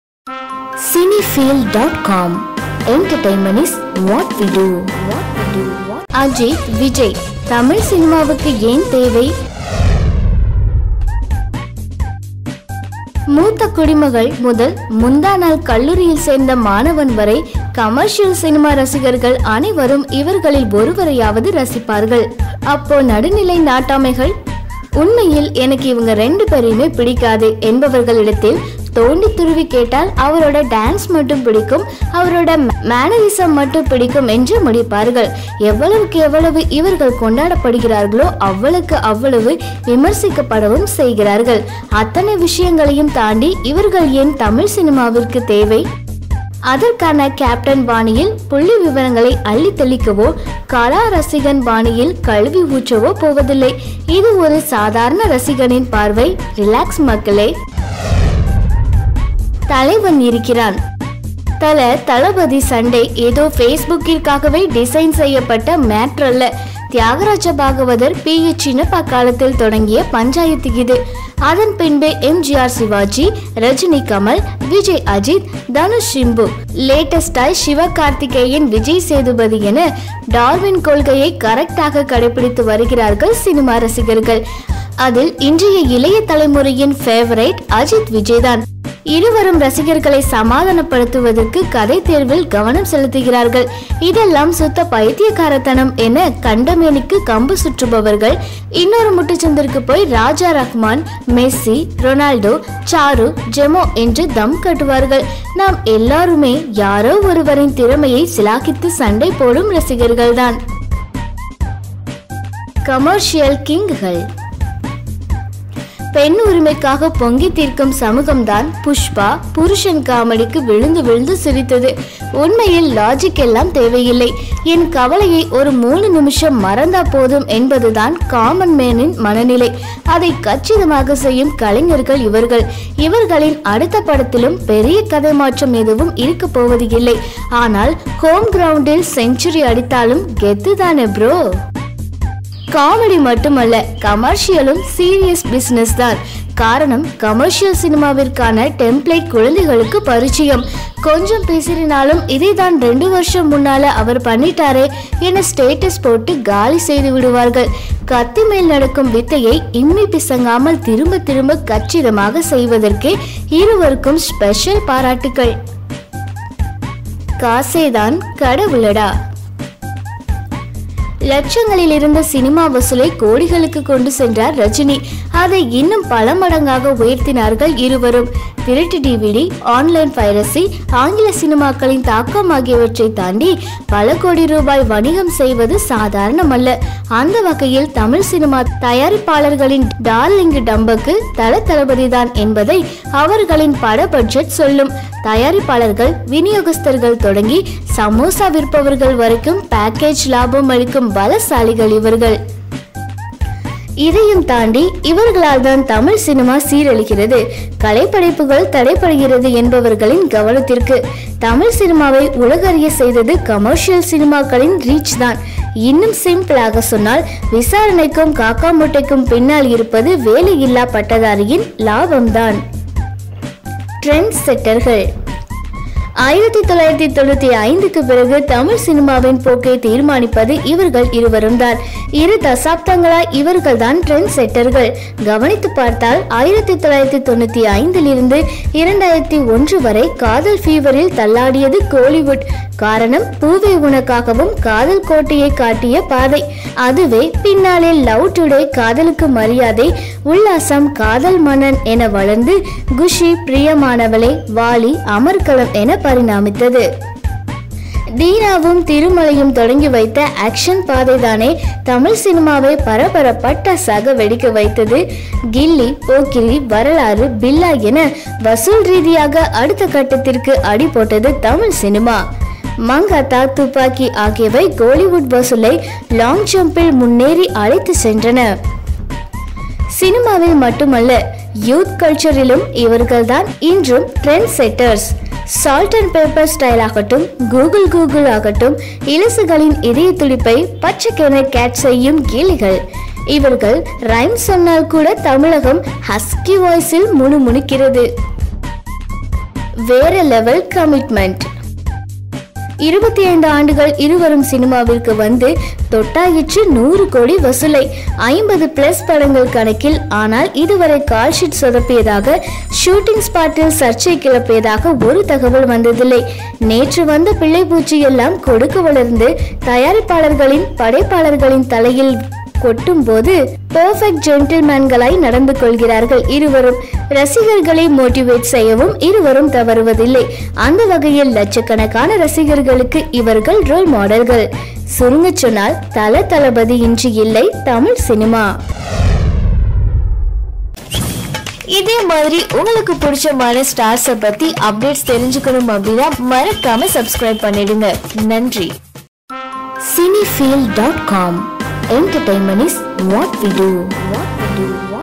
அஜித்-விஜய் தமிழ் சினிமாவுக்கு ஏன் தேவை..? - Cine Filed
Why Tamil Cinema Needs Ajith and Vijay
அஜித்-விஜய் தமிழ் சினிமாவுக்கு ஏன் தேவை..?